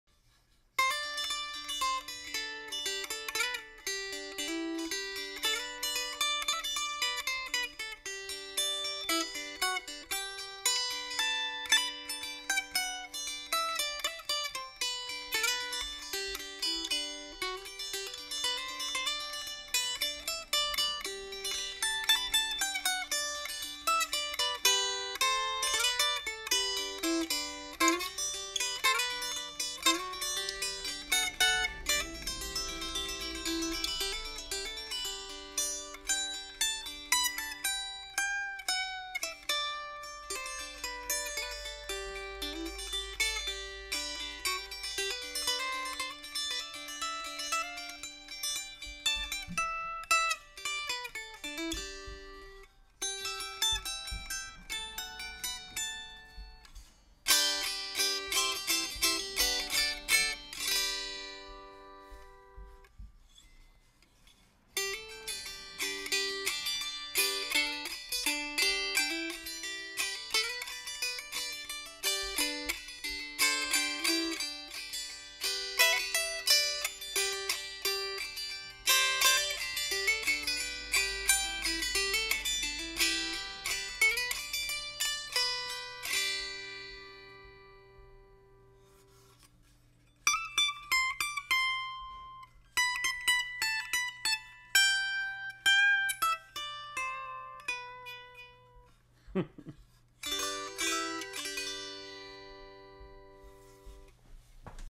1974 R. Caroprese piccolo banjola (5-string banjo)
This strange creature is a piccolo banjo but with a wood top and body and neck cut from a single piece of wood. I’m not sure what that makes it — maybe a “piccolo banjola?”

It’s marked R. Caroprese, Elmhurst (NY) 1974. I got this from “Banjo Dave” in trade and it’s just overall peculiar.

After a fret level/dress, new 5th peg, cleaning, and bridge, it plays spot-on but has a ginormous neck and not a lot of sound. It’s definitely a “backpack stuffer” and has an autoharp-ish, door harp-ish voice.